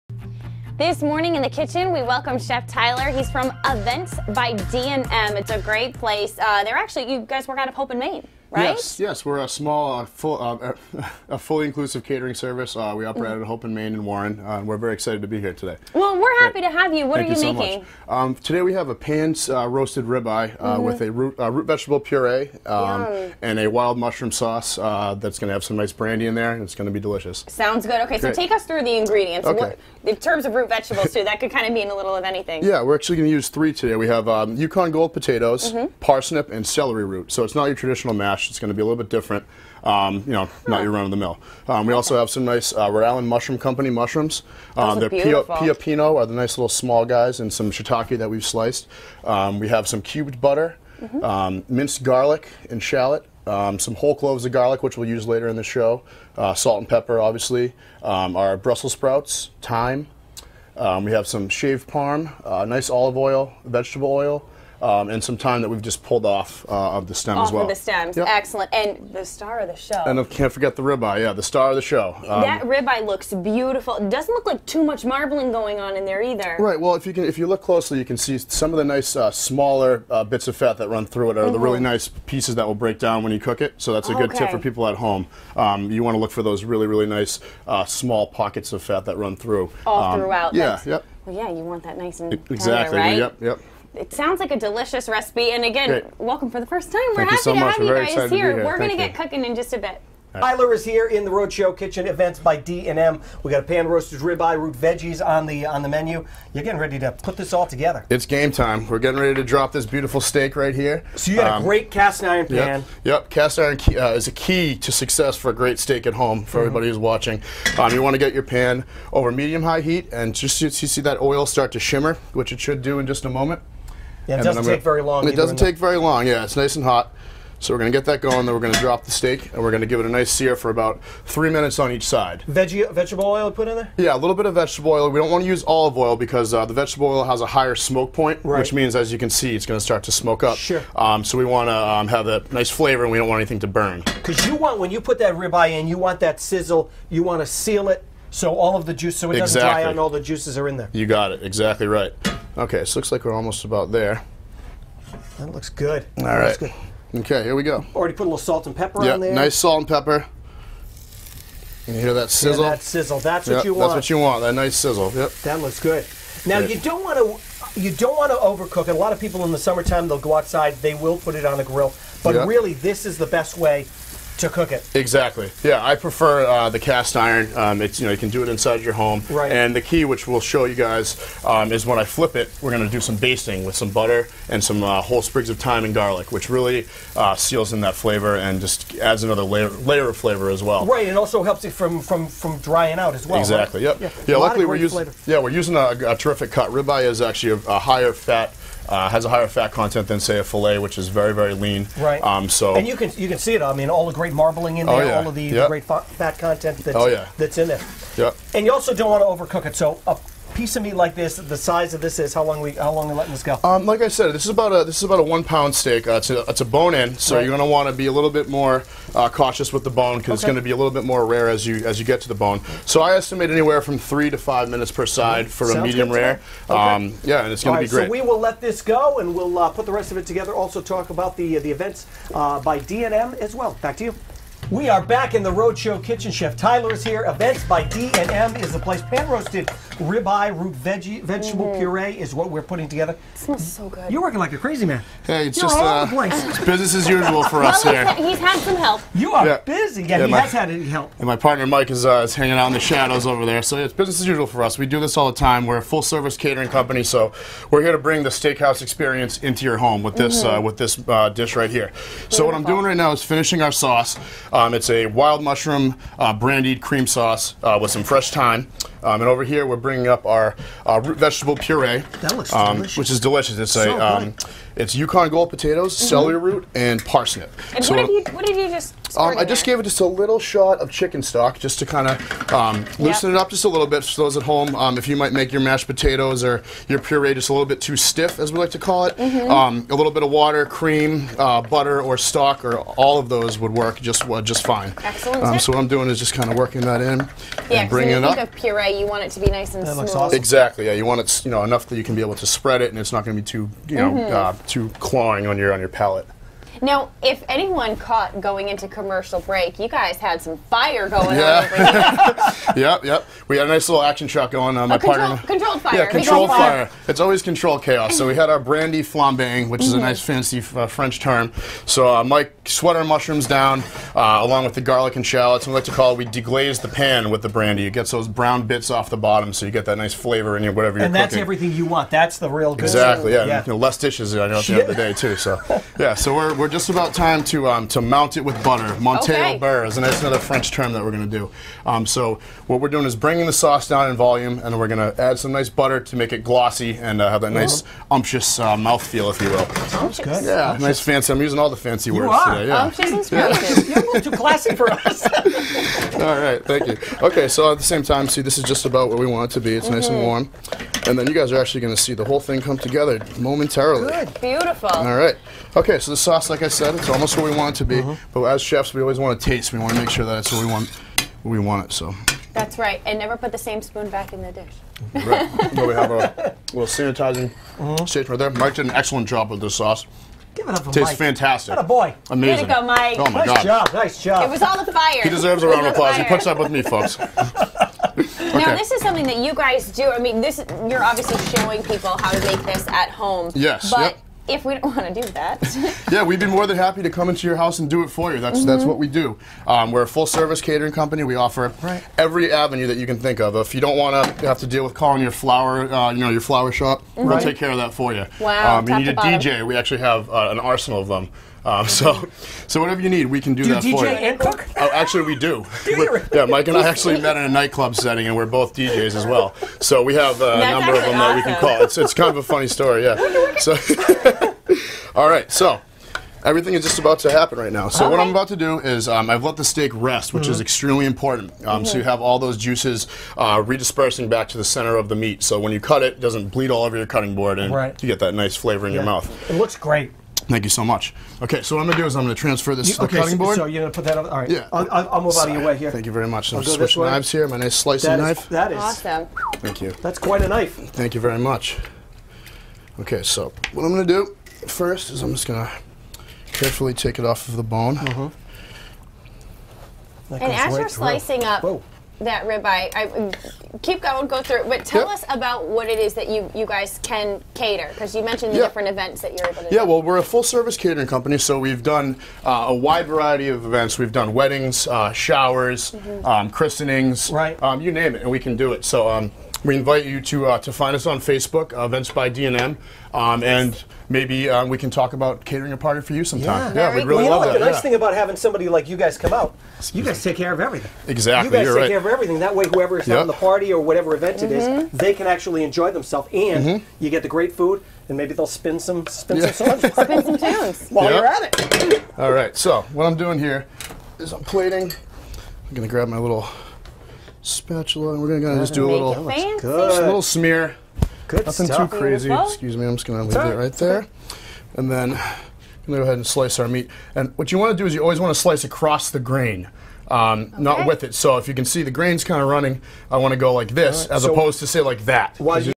In the Kitchen: Pan Roasted Rib Eye
Chef Tyler Doktor and Chef Michael Mancuso of Events by D&M demonstrates how to prepare Pan Roasted Rib Eye with root vegetable puree, roasted brussels, and brandied mushrooms (serves 2).